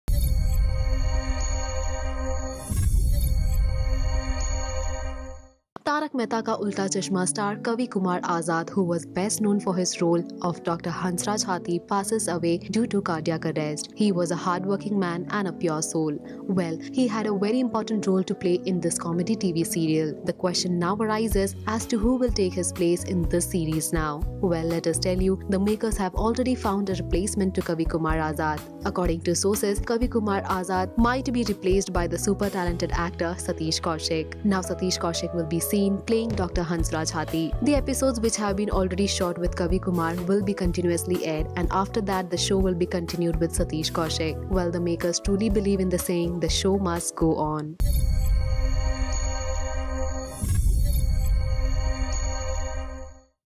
Now This Actor Will Replace Doctor Haathi In Taarak Mehta Ka Ooltah Chashmah
Tarak Mehta Ka Olta Chashma star Kavi Kumar Azad, who was best known for his role of Dr. Hansraj Haathi passed away due to cardiac arrest. He was a hardworking man and a pure soul. Well, he had a very important role to play in this comedy Tv serial. The question now arises as to who will take his place in this series now. Well, let us tell you the makers have already found a replacement to Kavi Kumar Azad. According to sources, Kavi Kumar Azad has been replaced by the super talented actor Satish Kaushik. Yes! now Satish Kaushik will be seen playing Dr. Hansraj Hatthi. Well, the episodes which have been already shot with Kavi Kumar will be continuously aired and after that, the show will be continued with Satish Kaushik. Well, the makers truly believe in the saying," The Show Must Go On."

Category
Entertainment
License